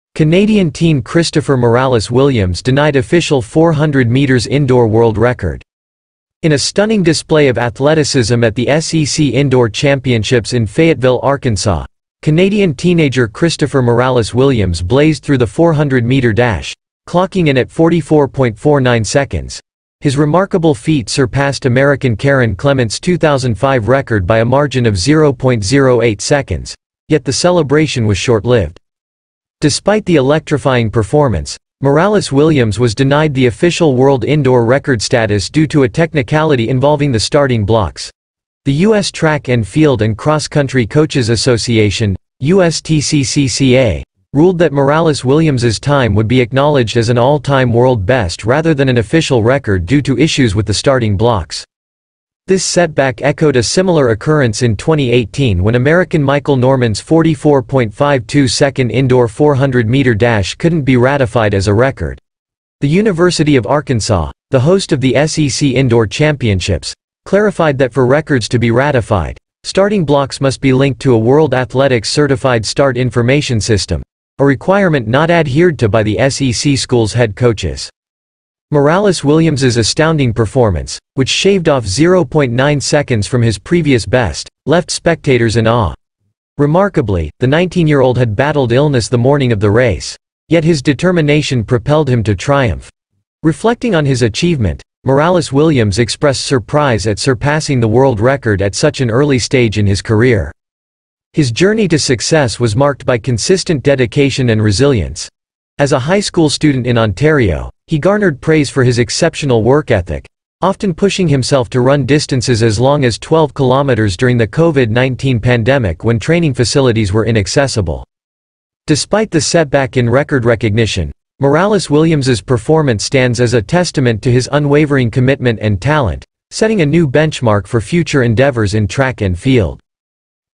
Canadian teenager Christopher Morales Williams denied 400m indoor world record due to starting block
**Canadian Teen Christopher Morales Williams Denied Official 400m Indoor World Record**

In a stunning display of athleticism at the SEC Indoor Championships in Fayetteville, Arkansas, Canadian teenager Christopher Morales Williams blazed through the 400-meter dash, clocking in at 44.49 seconds. His remarkable feat surpassed American Kerron Clement’s 2005 record by a margin of 0.08 seconds, yet the celebration was short-lived.

Despite the electrifying performance, Morales Williams was denied the official world indoor record status due to a technicality involving the starting blocks. The US Track and Field and Cross Country Coaches Association (USTCCCA) ruled that Morales Williams' time would be acknowledged as an all-time world best rather than an official record due to issues with the starting blocks.

This setback echoed a similar occurrence in 2018 when American Michael Norman's 44.52-second indoor 400-meter dash couldn’t be ratified as a record. The University of Arkansas, the host of the SEC Indoor Championships, clarified that for records to be ratified, starting blocks must be linked to a World Athletics-certified Start Information System, a requirement not adhered to by the SEC schools' head coaches.

Morales Williams' astounding performance, which shaved off 0.9 seconds from his previous best, left spectators in awe. Remarkably, the 19-year-old had battled illness the morning of the race, yet his determination propelled him to triumph. Reflecting on his achievement, Morales Williams expressed surprise at surpassing the world record at such an early stage in his career.

His journey to success was marked by consistent dedication and resilience. As a high school student in Ontario, he garnered praise for his exceptional work ethic, often pushing himself to run distances as long as 12 kilometers during the Covid-19 pandemic when training facilities were inaccessible.

Despite the setback in record recognition, Morales Williams' performance stands as a testament to his unwavering commitment and talent, setting a new benchmark for future endeavors in track and field.